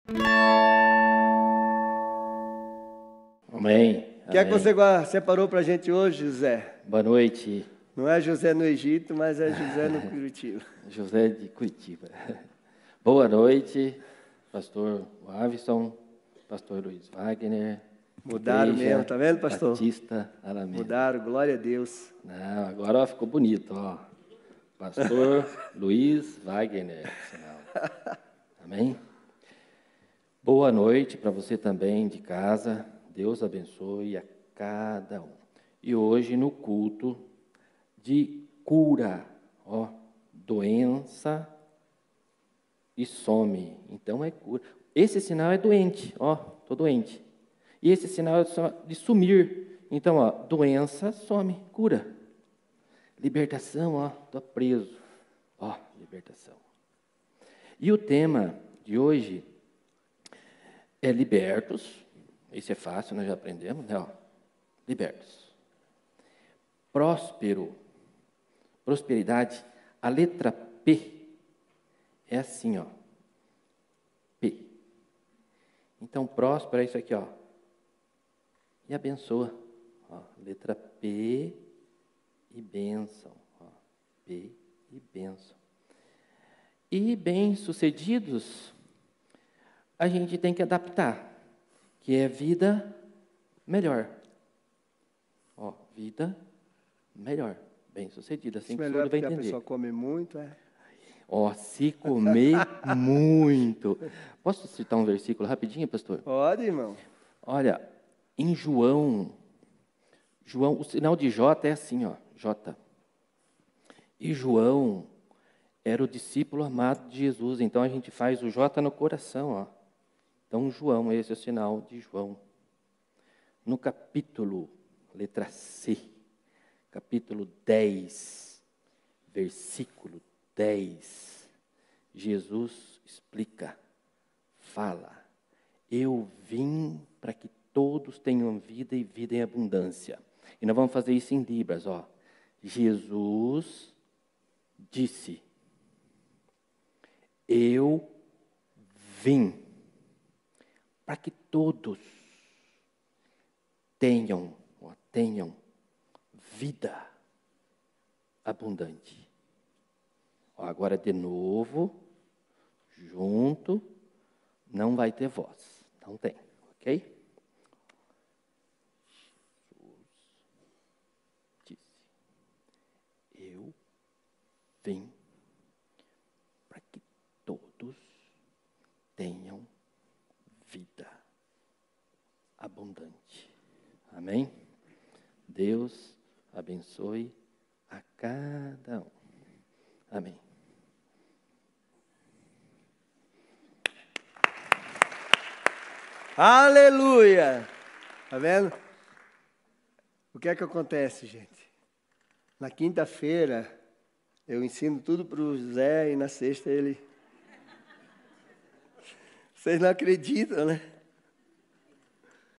MINUTO LIBRAS | 23Jun2023 | Libertos Prósperos e Bem-Sucedidos
Meus amados,
É com grande prazer que damos seqüencia ao 'Minuto Libras' aqui no canal LouvandoSempre. Toda sexta feira um dos nossos intérpretes repassam o tema do culto aos presentes no templo da Igreja Batista Alameda em Curitiba. Venham conosco aprender um pouco dessa língua. Abençoar aos nossos irmãos surdos e ampliar os nossos relacionamentos.